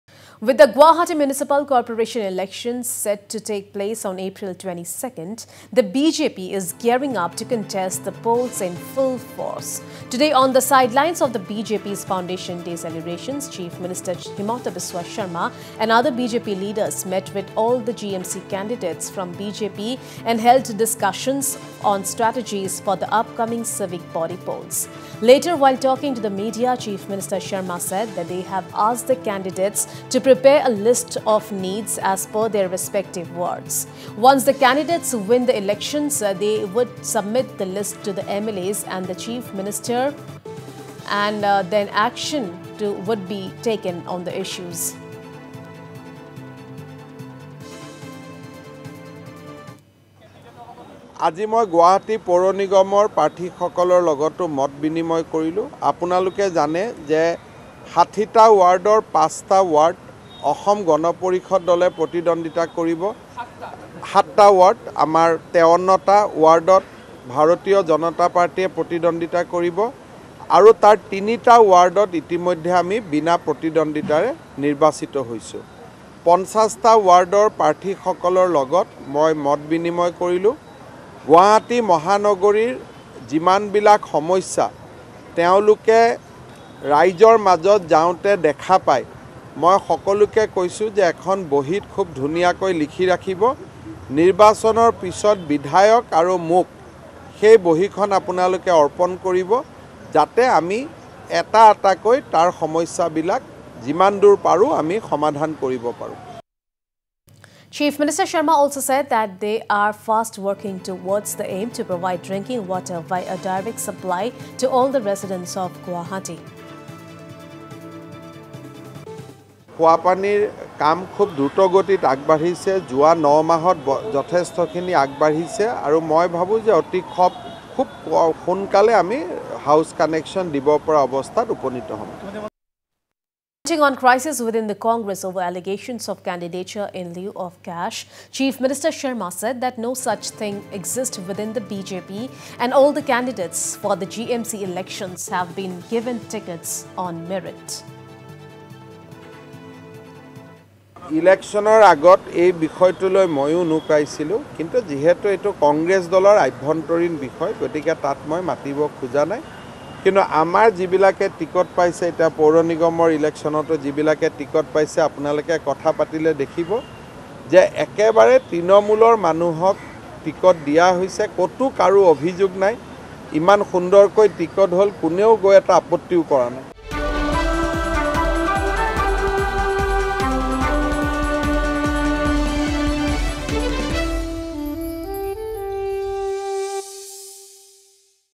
BJP gearing up for Guwahati Municipal Corporation polls
Assam | With the GuwahatiMunicipalCorporation elections set to take place on April 22, the BJP is gearing up to contest the polls in full force.

Today on the sidelines of the BJP's foundation day celebrations, Chief Minister Dr.HimantaBiswaSarma and other BJP leaders met with all the GMC candidates from BJP and held discussions on strategies for the upcoming civic body polls.